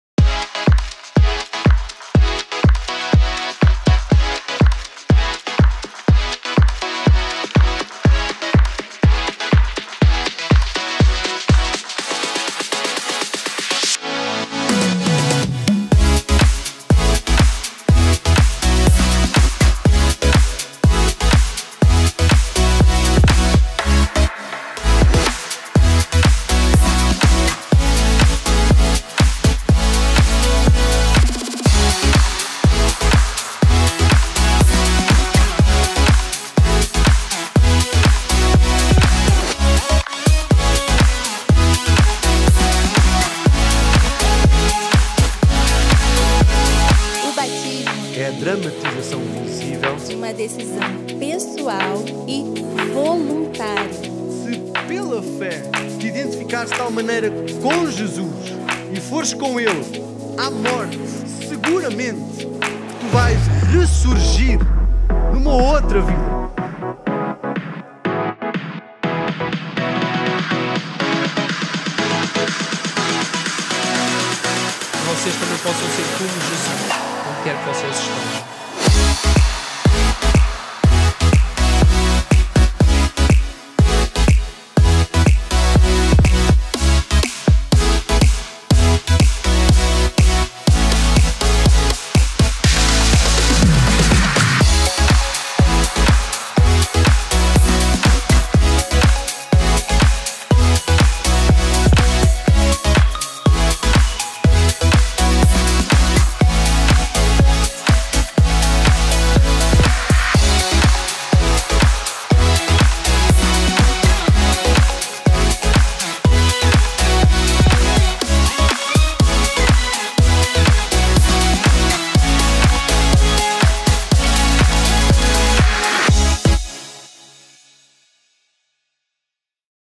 Esta é a minha Igreja!
Recap da celebração especial de batismos no dia 20 Jun 2021.